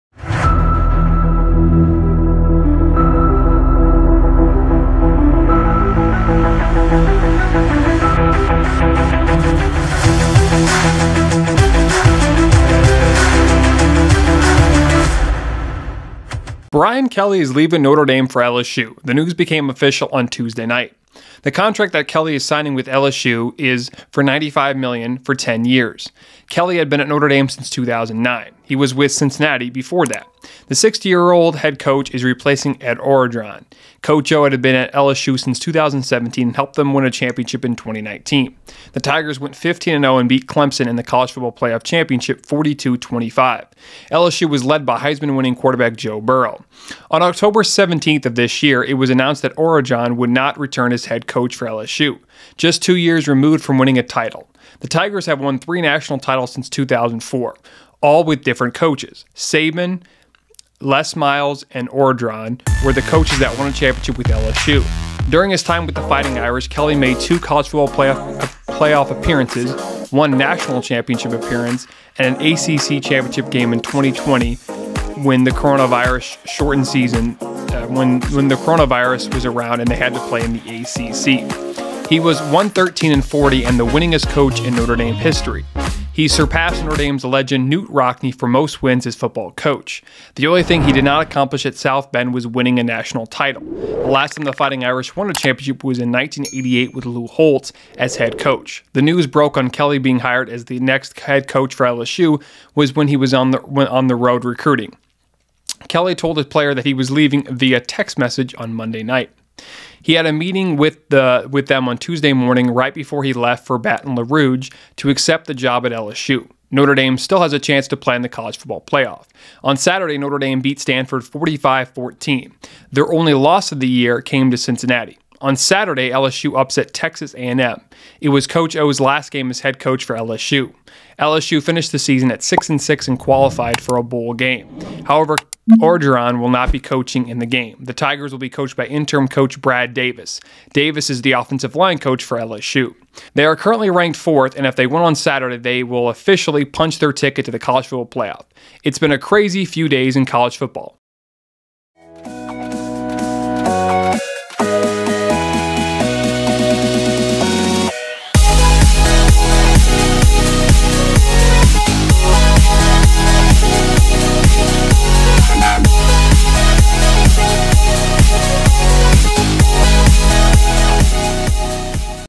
Breaking News: Brain Kelly is leaving Notre Dame for LSU!
Brian Kelly has decided to leave Notre Dame for the LSU Tigers!

No politics, no drama and no arguing. Just a guy talking  about sports 24/7. A weekly podcast with JJ Peters.